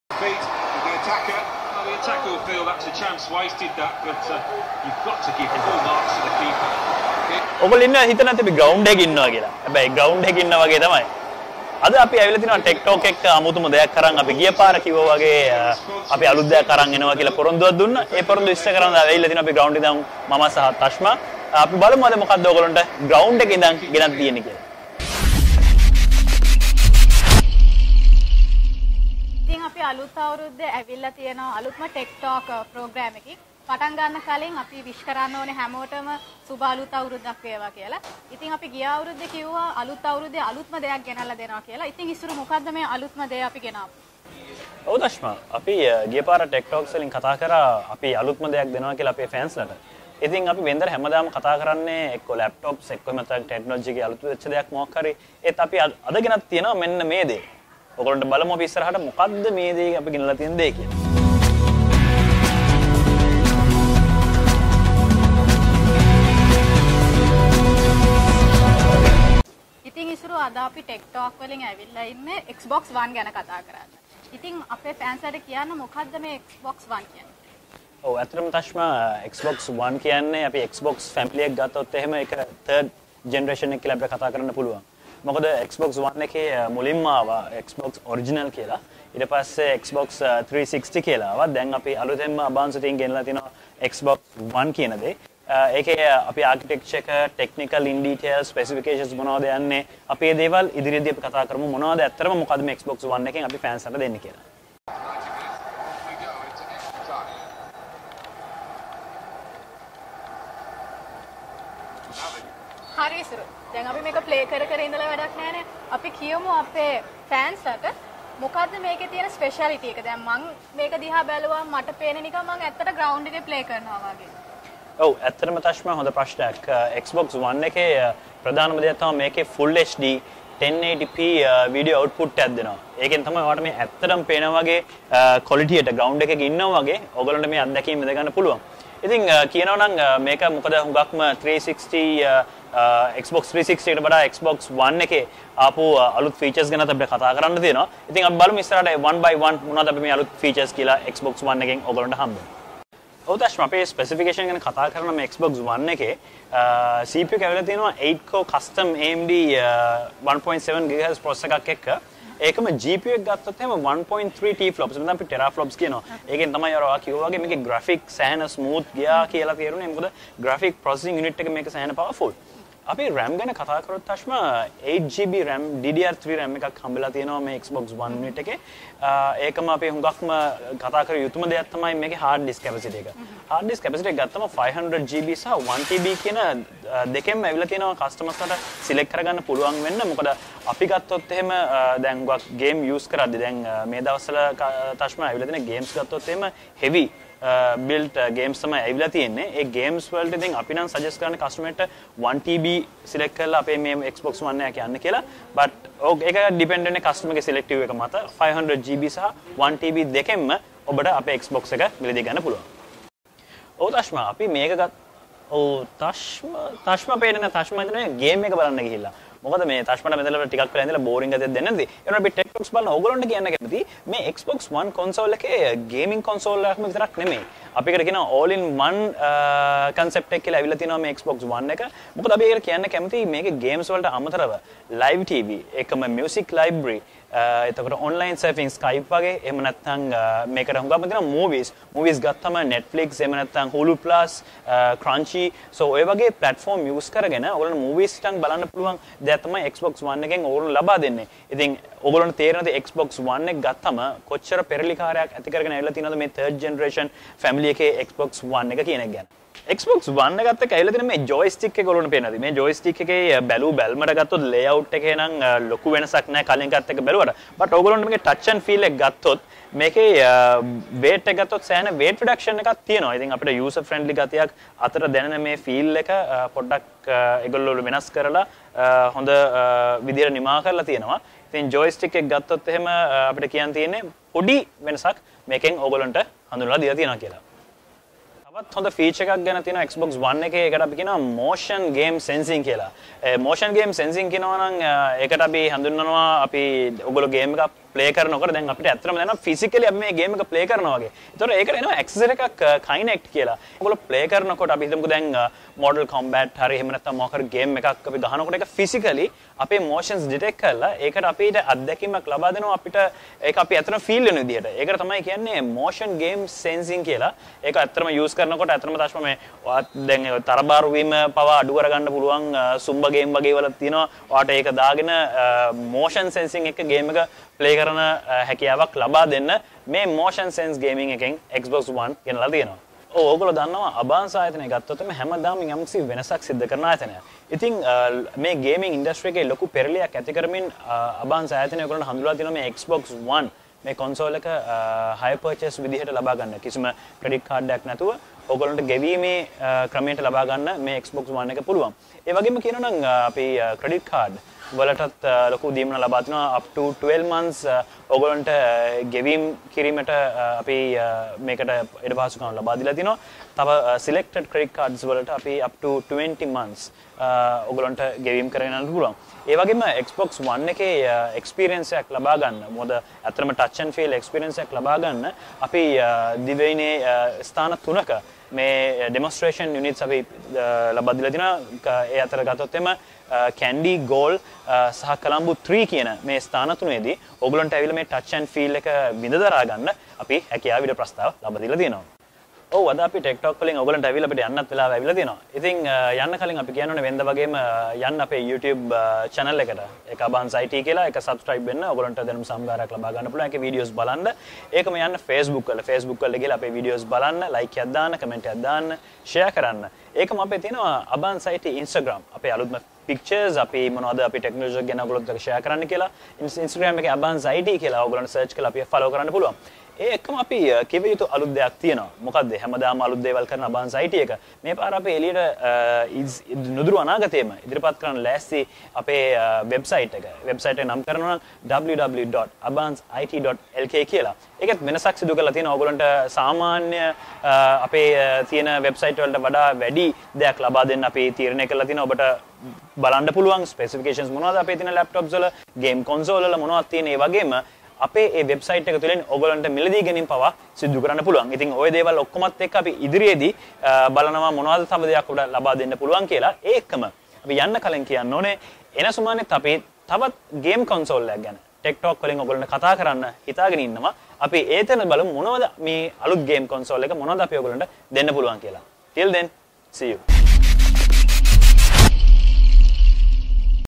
Tech Talk (Xbox One Gaming Console)
Xbox One: Featuring a 500GB or 1TB hard drive, a wireless controller, backward compatibility for an ever-expanding number of Xbox 360 games, and the power to access your favorite apps directly from your gaming console, it puts your favorite entertainment right at your fingertips.